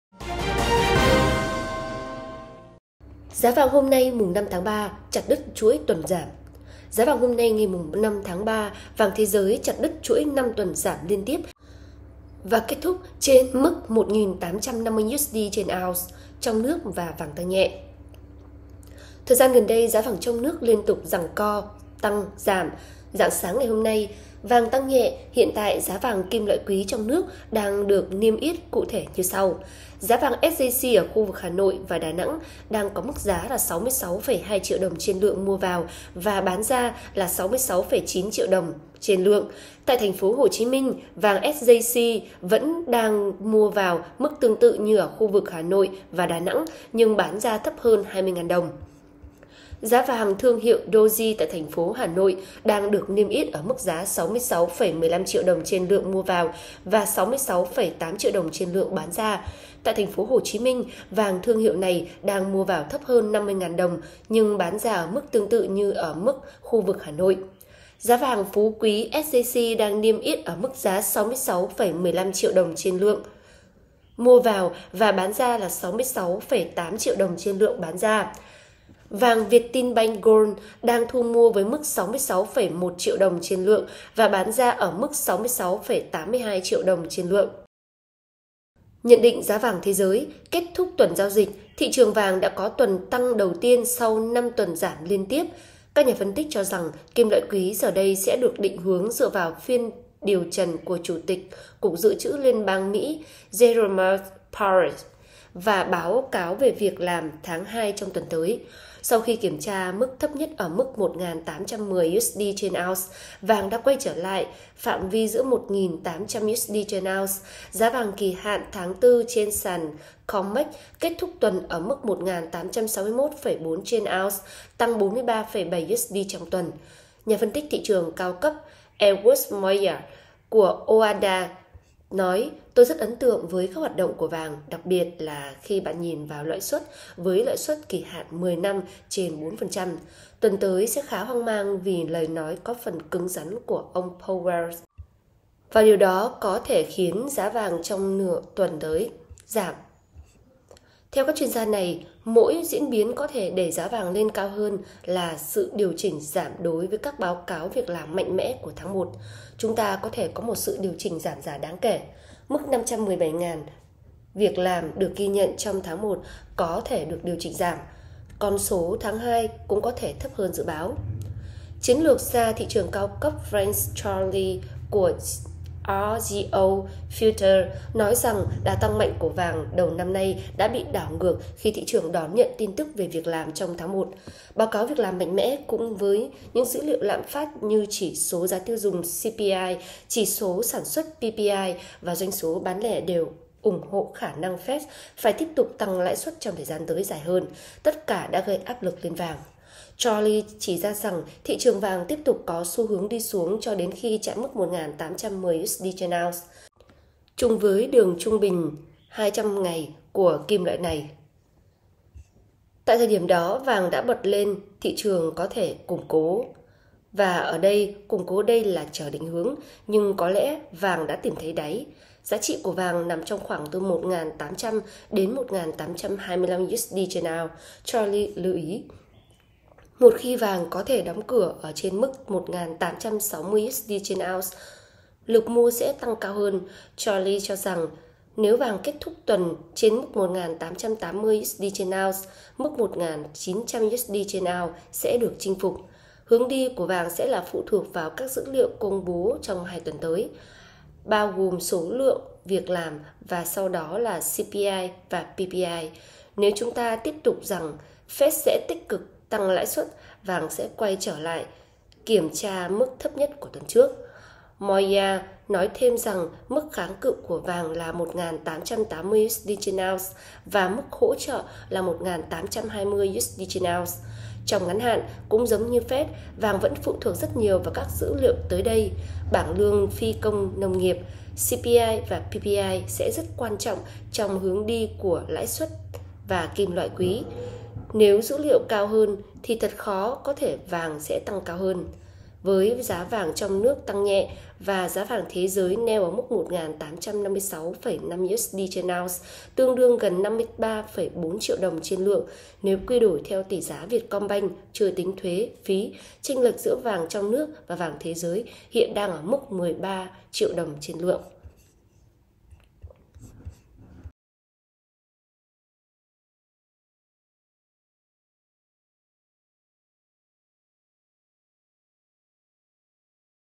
Giá Vàng Ngày 5.3: “Chặt đứt” chuỗi tuần giảm
Giá vàng hôm nay (5-3): Vàng thế giới “chặt đứt” chuỗi 5 tuần giảm liên tiếp và kết thúc trên mức 1.850 USD/ ounce. Trong nước, vàng tăng nhẹ.

Giá vàng trong nước hôm nay

Thời gian gần đây, giá vàng trong nước liên tục giằng co tăng – giảm. Rạng sáng hôm nay, vàng tăng nhẹ. Hiện tại, giá kim loại quý trong nước đang được niêm yết cụ thể như sau:

Giá vàng SJC ở khu vực Hà Nội và Đà Nẵng đang là 66,2 triệu đồng/lượng mua vào và 66,92 triệu đồng/lượng bán ra. Tại TP Hồ Chí Minh, vàng SJC vẫn đang mua vào mức tương tự như ở khu vực Hà Nội và Đà Nẵng nhưng bán ra thấp hơn 20.000 đồng.